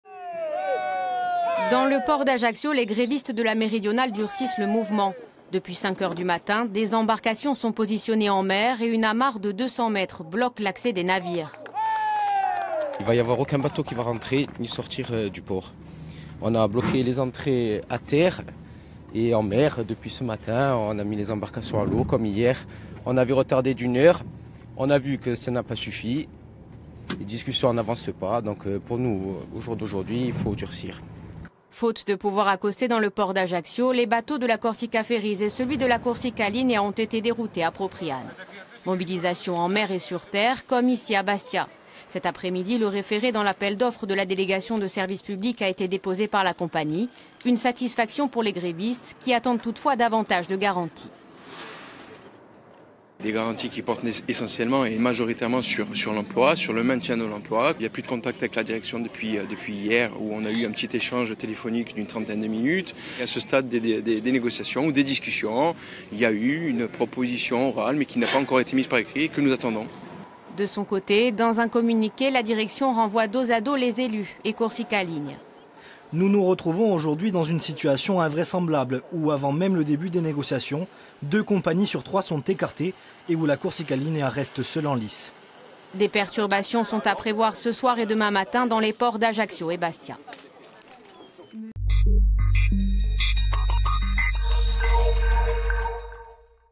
Corse : quatrième jour d'action pour les marins grévistes de la Méridionale
Ce jeudi 28 février, des grévistes de la Méridionale ont bloqué des bateaux d’autres compagnies dans le port d’Ajaccio. À Bastia, le port a été débloqué mercredi. La reconduction de la grève a été votée par les adhérents du STC.

Intervenants - Marc-Aurèle Orsoni, Délégué STC La Méridionale ; Cyril Venouil, Délégué STC La Méridionale.
Equipe - Marianne Romani ; Stéphane Lapera ; Marion Fiamma ; Jacques Paul-Stefani ; Frédéric Guichard.